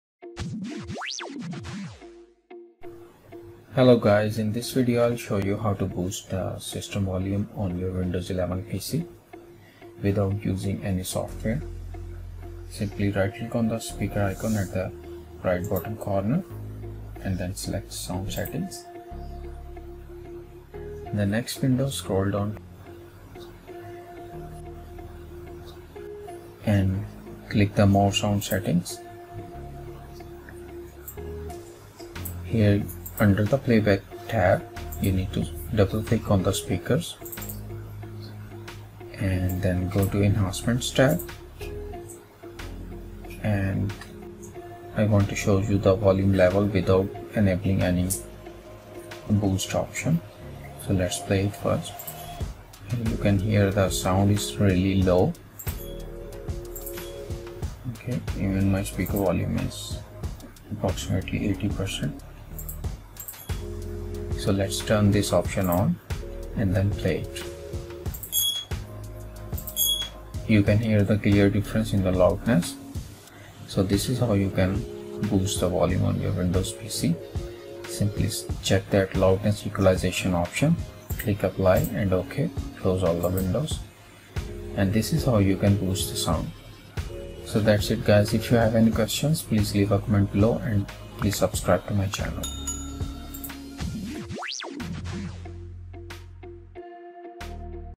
Boost Speaker Volume in Windows 11/10 Without Using Any Software
My Speaker volume is moderate and sometimes when I am in a noisy place then barely hear the sound from my computer's speakers. Instead of using an external speaker, I found a setting in Windows that I used to BOOST the sound of my speaker even the volume was already at maximum settings on my Windows PC.

In this guide, you will learn how to increase the volume of Windows PC without using any software. You can boost the volume of your speaker to approx 200%.